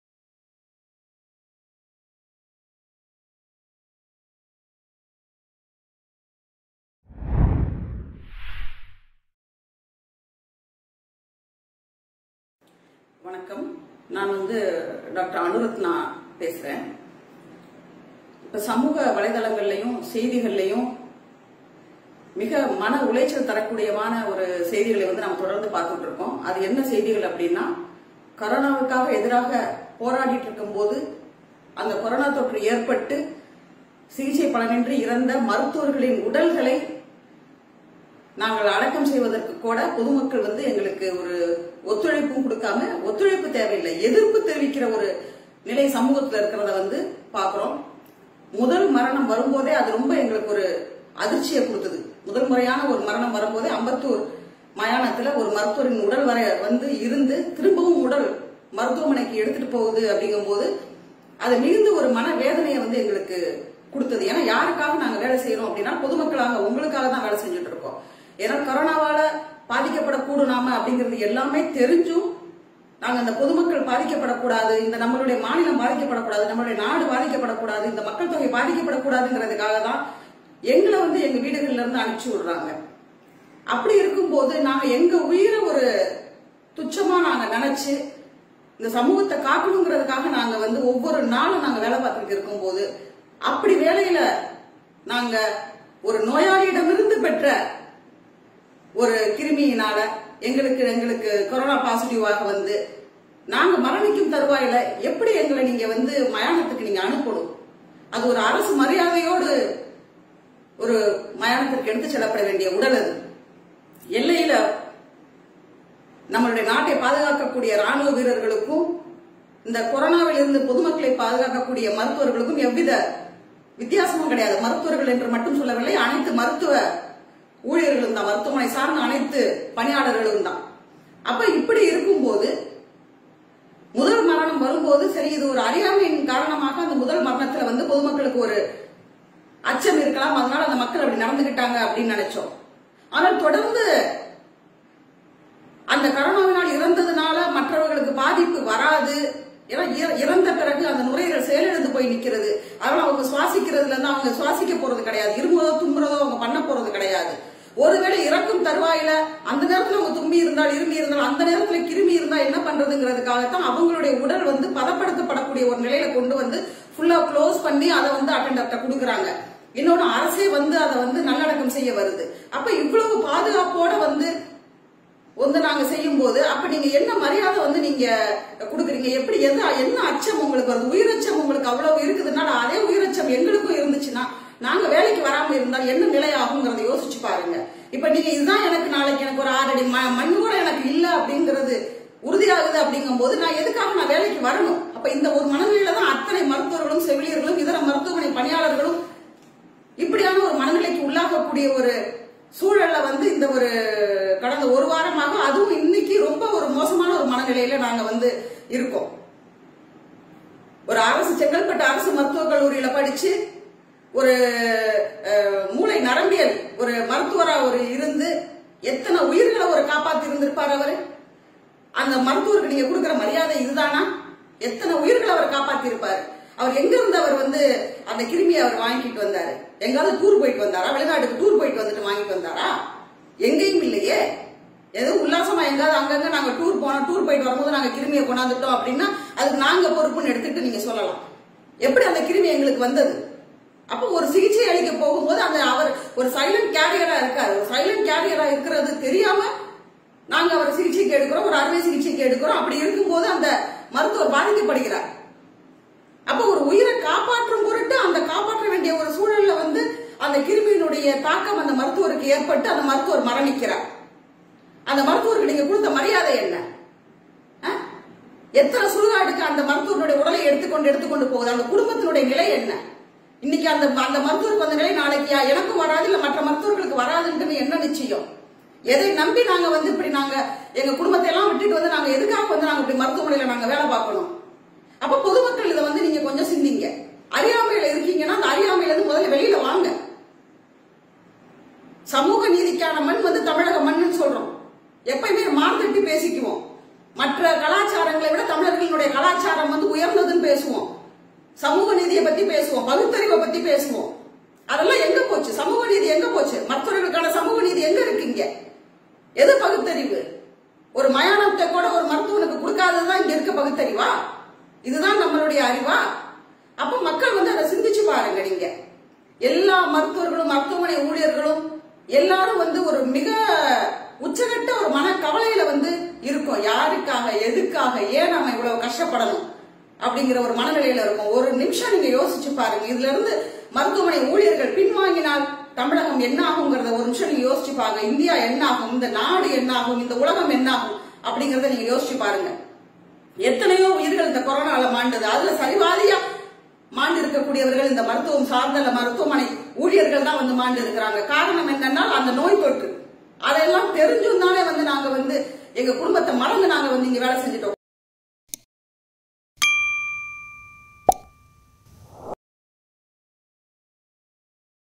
மயானத்தை தர மறுத்த பொதுமக்கள் வருத்தப்படும் மருத்துவர்கள்.. Dr Anu Ratna Speech / Dr Simon Hercules
மயானத்தை தர மறுத்த பொதுமக்கள் வருத்தப்படும் மருத்துவர்கள்.. Dr_AnuRatna Speech /